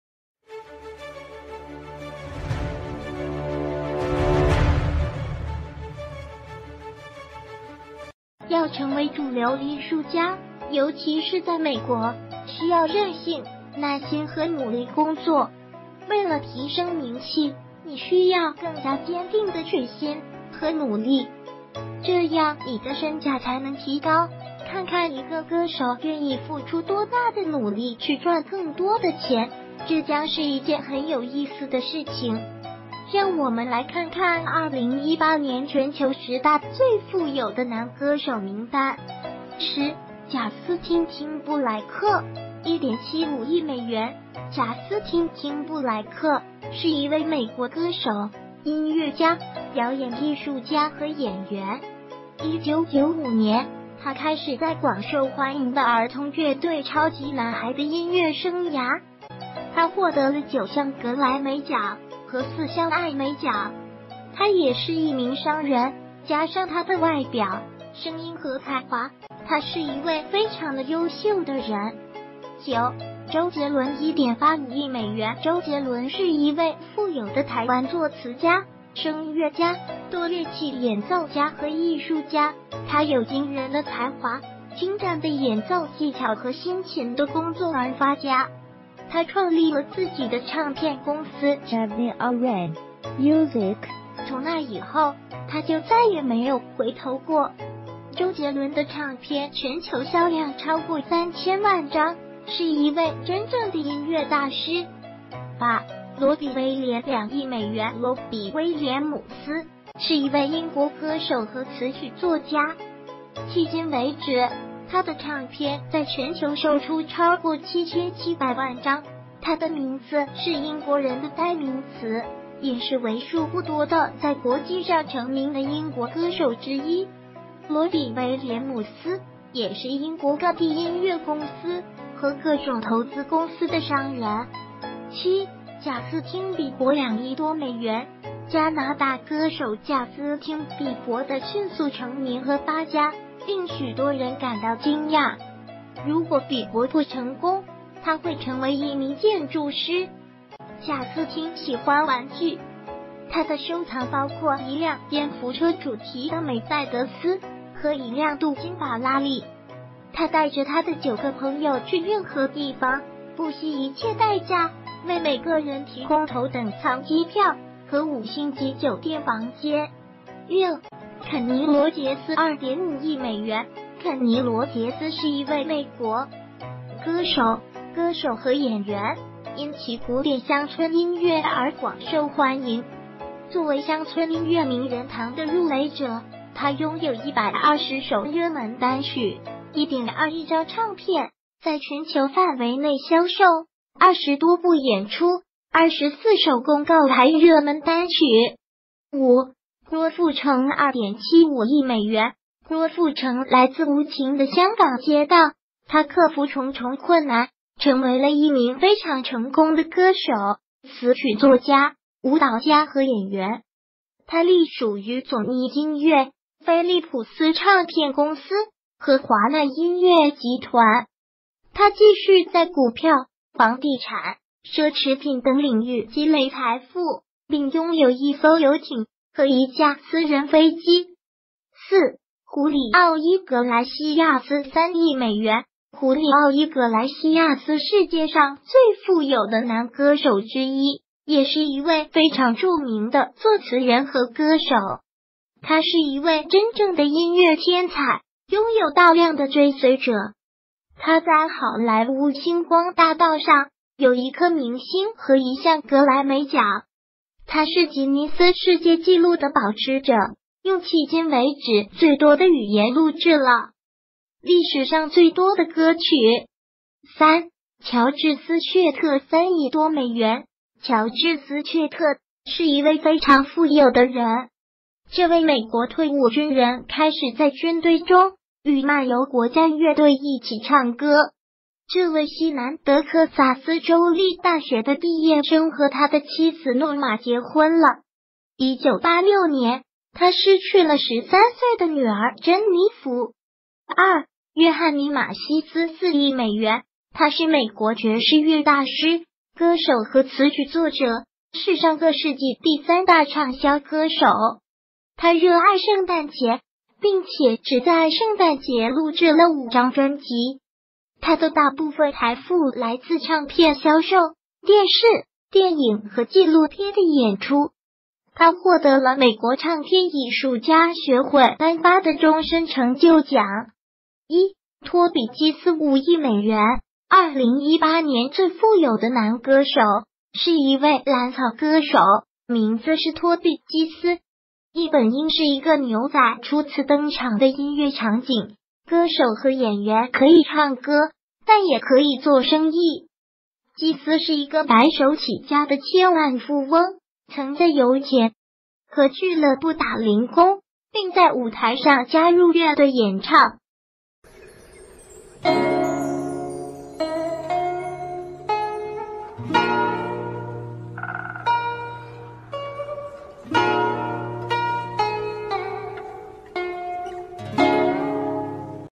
世界十大最富有的男歌手 其中有兩位中國歌手
要成為主流藝術家，尤...